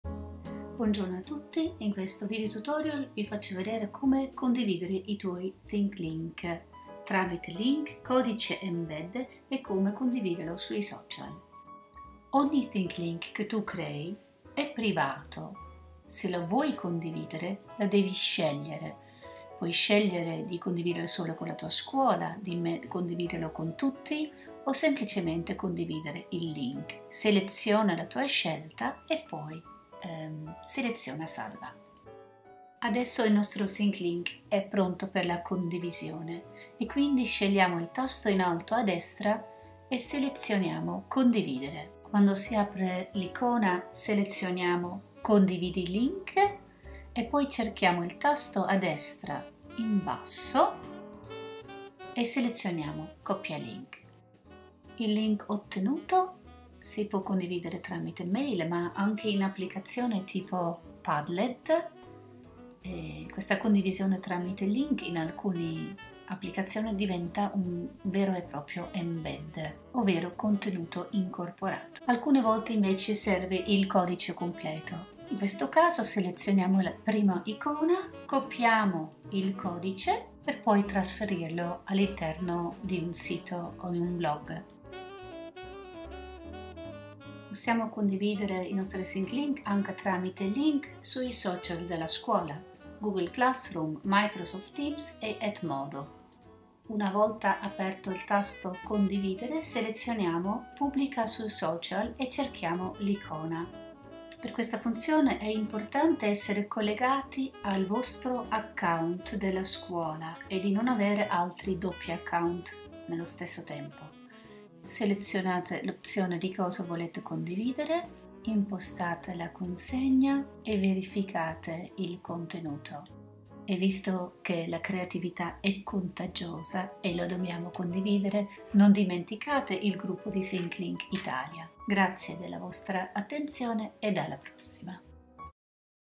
Condividere un Thinglink
Come e dove  condividere i tuoi  Thinglink. Esplora la possibilità di condividere direttamente un Thinglink in Google Classroom, Edmodo e Microsoft Teams.